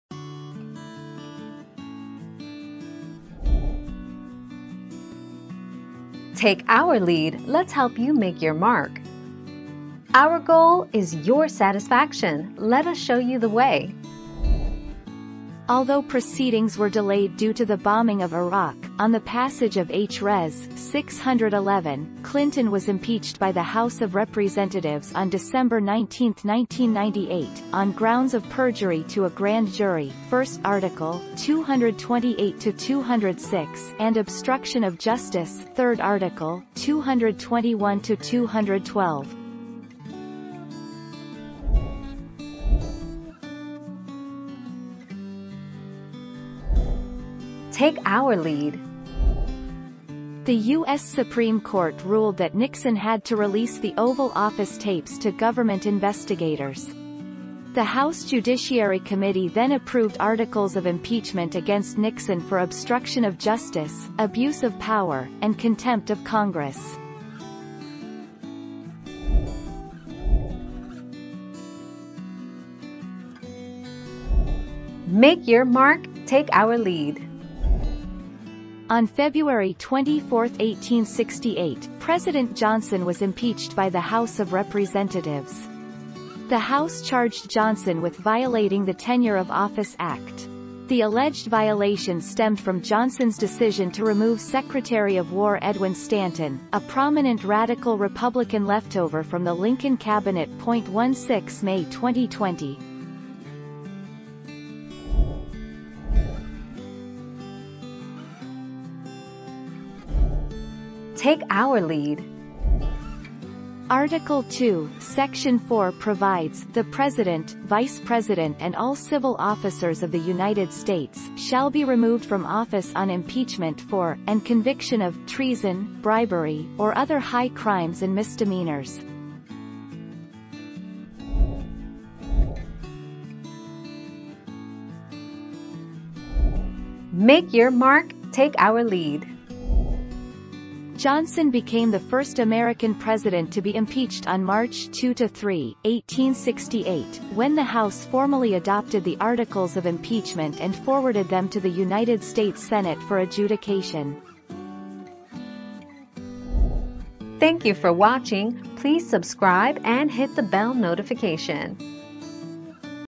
Was Clinton impeached?
00:00 - Was Clinton impeached?
00:43 - Why was Nixon impeached?
01:11 - Why was Johnson impeached?
01:46 - Can a president be removed by impeachment?
02:12 - Who first impeached president?

Laura S. Harris (2021, March 2.) Was Clinton impeached?
    AskAbout.video/articles/Was-Clinton-impeached-226412